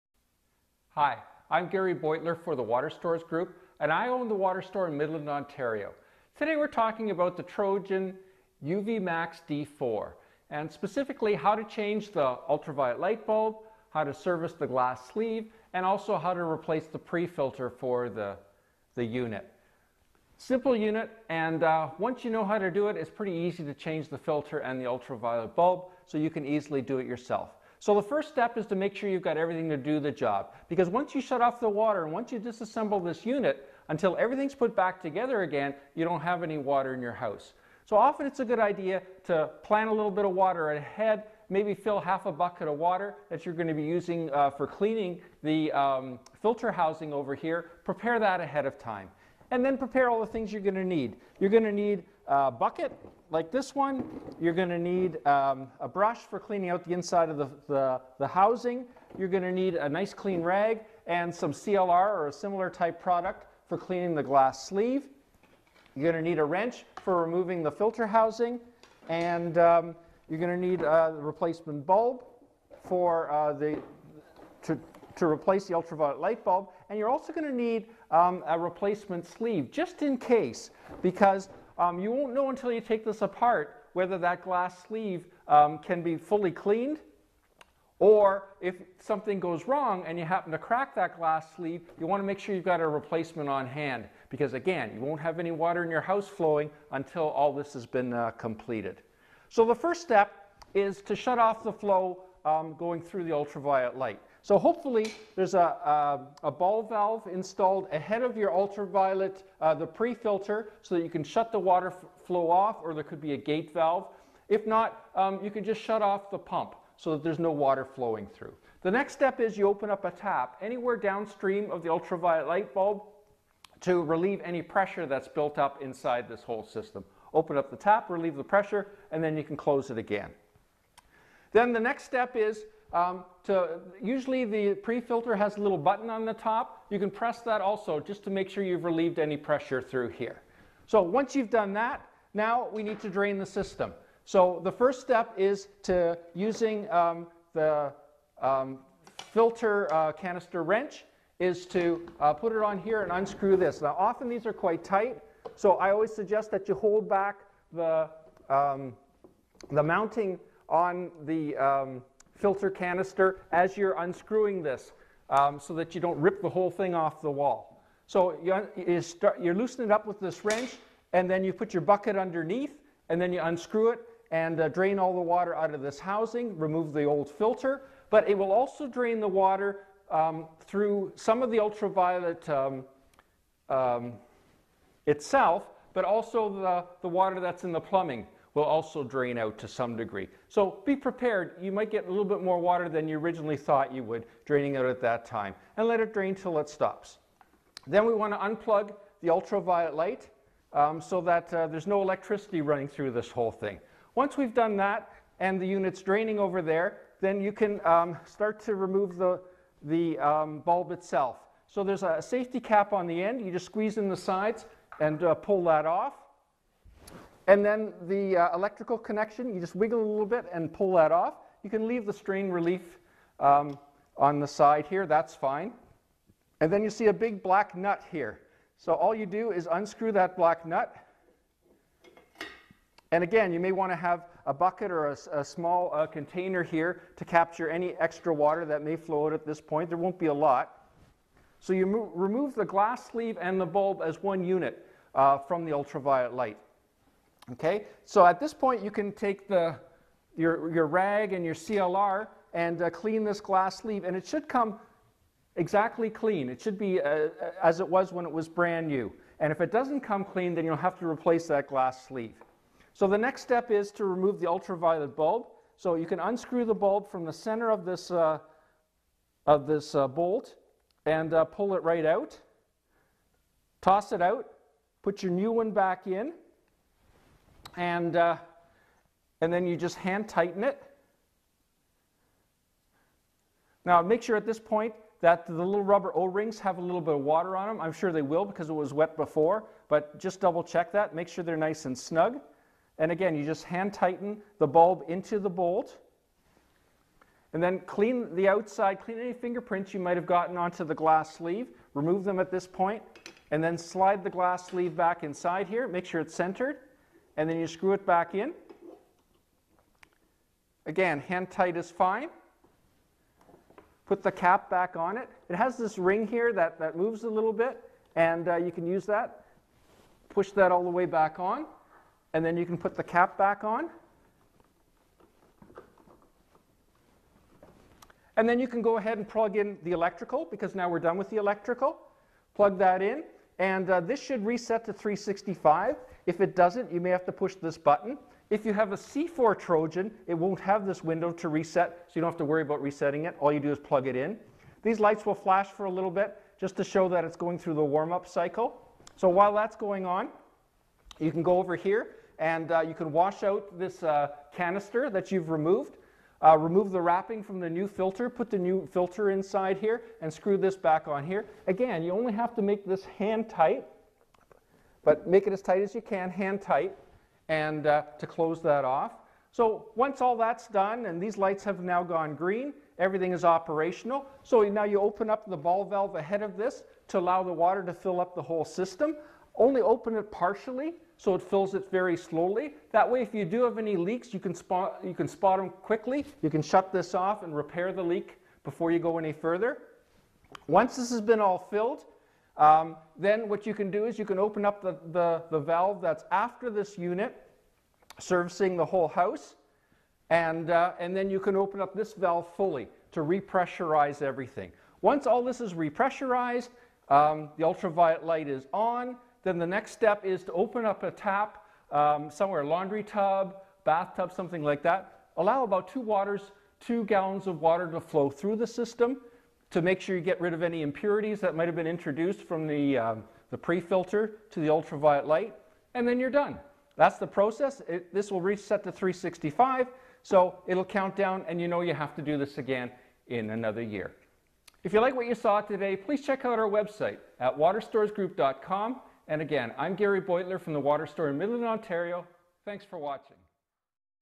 How to Change the Lamp in Viqua/Trojan UV Max Ultraviolet System
Get a FREE REPORT on the 10 Biggest Mistakes made when purchasing water treatment equipment and how

This video by Gary the Water Guy discusses why How to change the bulb in a Trojan UV Max ultraviolet water disinfection system.
Trojan home Ultra Violet Disinfection Systems have been around for years and with a little guidance you can change your own filter, UV bulb and clean the glass sleeve yourself.  Caution: you will not be able to use any water in your house until this process has been completed, so before you start, plan accordingly.

1) The first step is to make sure you have everything ready.  You will need a bucket, a clean cloth, some CLR or a similar cleaning product, the correct size replacement filter and Ultra Violet bulb and a spare glass sleeve just in case the old one cannot be thoroughly cleaned or gets broken.

2) Turn off the water supply before the Ultra Violet pre filter and open a tap downstream of the UV unit to release the pressure and then close the tap.  Close a ball valve after the UV to shut-off the water to keep it from flowing backward through the UV.

3) Unplug the power supply and unplug the sensor (if you have one) from the blue jack. At this point you should let the system cool for 10 minutes or so.   Squeeze sides of the black cap at the end of the UV and remove.  Unplug power from bulb by pulling lamp plug from bulb.

4) Push red button on top of pre filter to relieve pressure from inside and unscrew the housing.  Be sure to put a bucket under this housing as you might get quite a bit of water flowing through the system as the piping and UV empty.

5) Unscrew the sleeve bolt and remove the bulb and sleeve as one unit.  Clean the sleeve with CLR or a similar product.  If the sleeve cannot be completely cleaned, you will need to replace the sleeve with a new one.

6) Unscrew the lamp from inside the sleeve bolt and replace with a new bulb.  When replacing the lamp/sleeve assembly, make sure that it is centered and the o rings are wet when screwing the assembly into the end of the UV chamber.  Do not over-tighten as you could break the sleeve.

7) If your UV has a sensor, unscrew it, clean it with CLR and re attach to your UV before connecting the power wire to the lamp.  Push on black safety cap.

8) Clean inside filter housing for pre filter and replace filter.  Close housing by tightening canister, usually hand tight is enough.

9) Plug in UV sensor (if you have one) and power supply.    If the power supply does not reset to 365 automatically, push the lamp timer reset button for 5 seconds to reset.

10)  Slowly refill system by opening ball valve ahead of pre filter and check for leaks.  Slowly open ball valve after UV.  Once fully pressurized, you can fully open the ball valves before and after the UV system.

11) Drain the first two gallons of water that comes through the filter and UV.

2) LET’S CONNECT!

3) CONTACT!

4) SHOP!